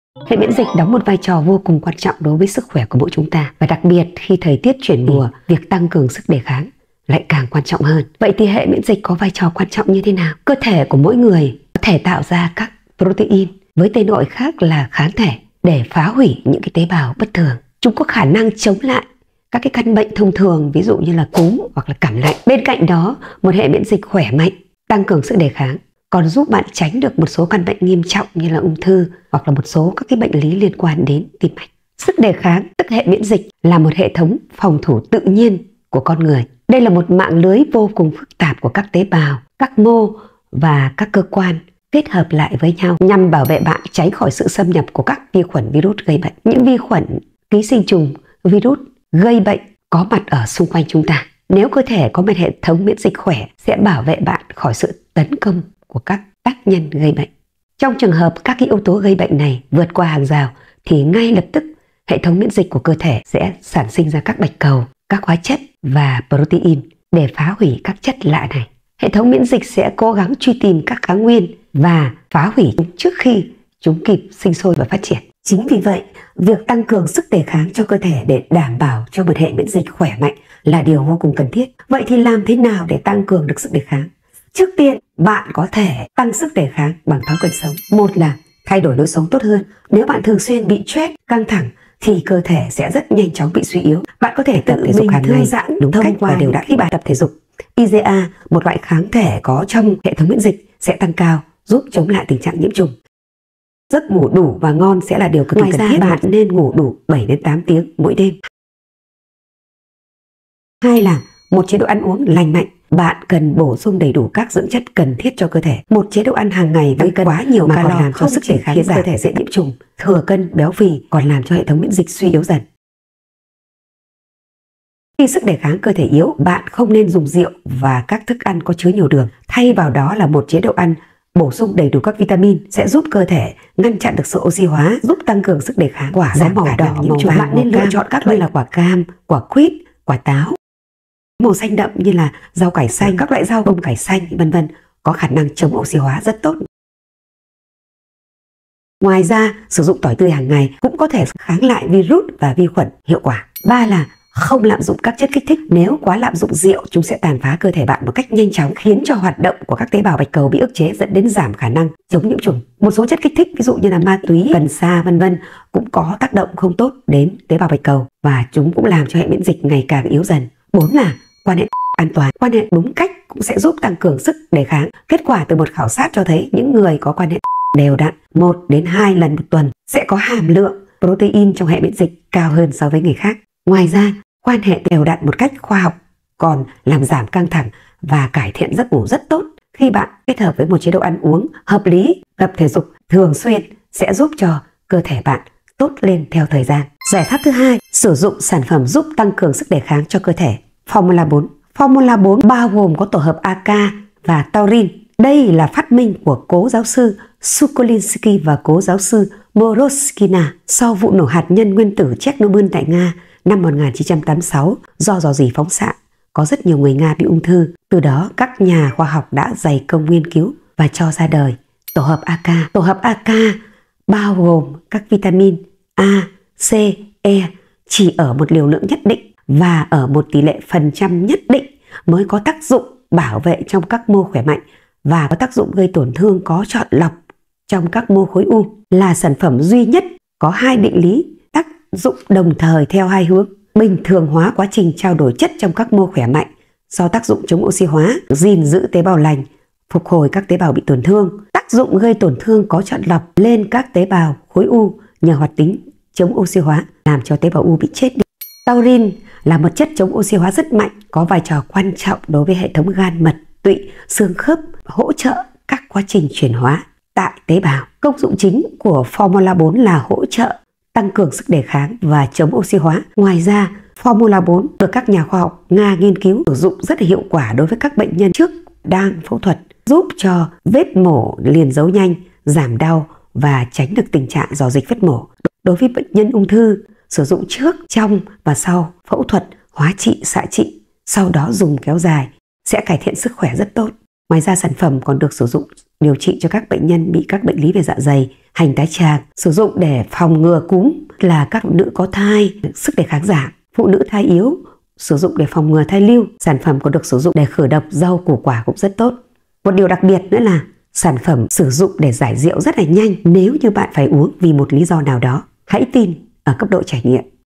GIẢI PHÁP TĂNG SỨC ĐỀ KHÁNG CHO CƠ THỂ
🌟 Sức đề kháng là một trong những vấn đề vô cùng quan trọng của cơ thể. Bên cạnh việc bổ sung đầy đủ dưỡng chất thì các hoạt động lành mạnh hàng ngày cũng là một yếu tố quan trọng để tăng cường sức đề kháng.

💌Trong video hôm nay BS Hoàn chia sẻ 2 giải pháp tăng sức đề kháng vô cùng hiệu quả mà bạn có thể áp dụng